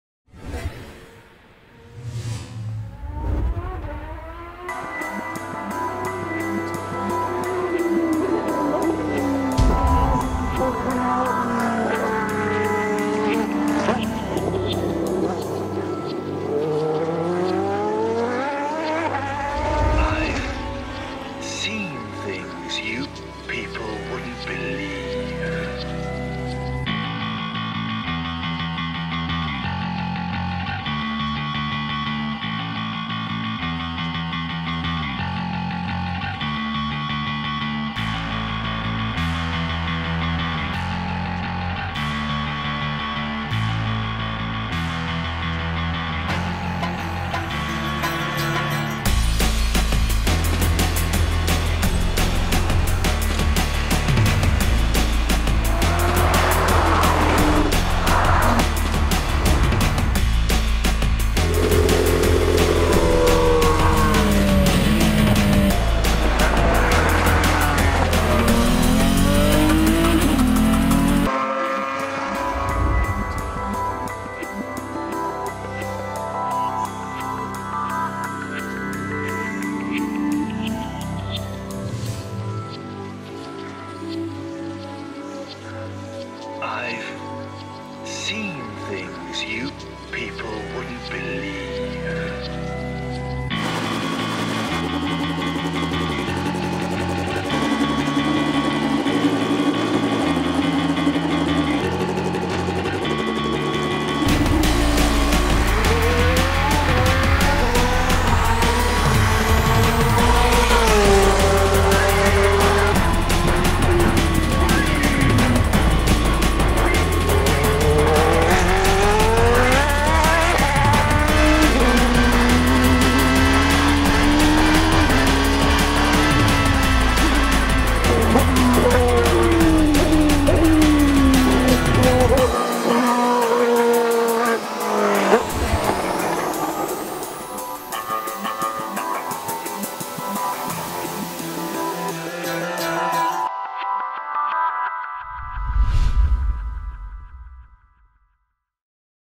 XP Développement Lotus F1 Circuit du Castellet 2011
Event VIP avec des Lotus Renault F1 2011

Coachs: Yannick Dalmas, Romain Grosjean et Xavier Pompidou